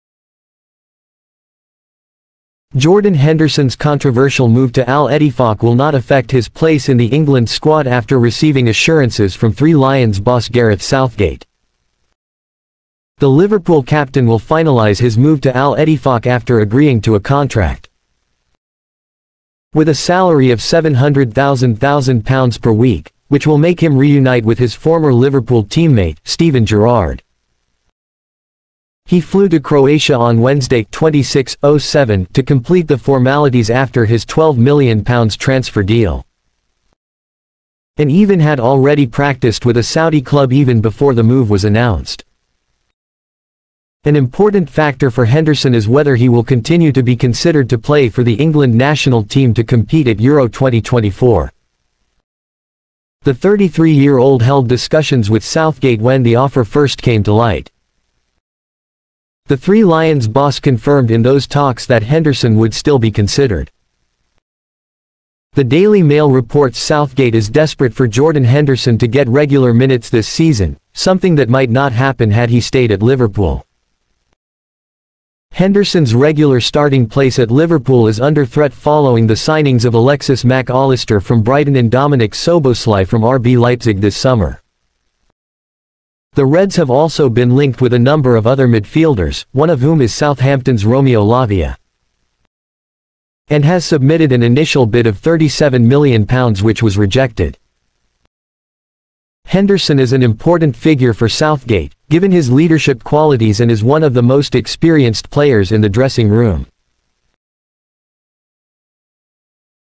Jordan Henderson's Controversial Move to Al-Ettifaq: What Does It Mean for England?
Jordan Henderson is set to complete his move to Al-Ettifaq, reuniting with his former Liverpool teammate Steven Gerrard. The move has been met with some controversy, with some questioning whether Henderson will still be considered for England. However, Gareth Southgate has confirmed that Henderson will still be in contention for the national team.

Jordan Henderson

Al-Ettifaq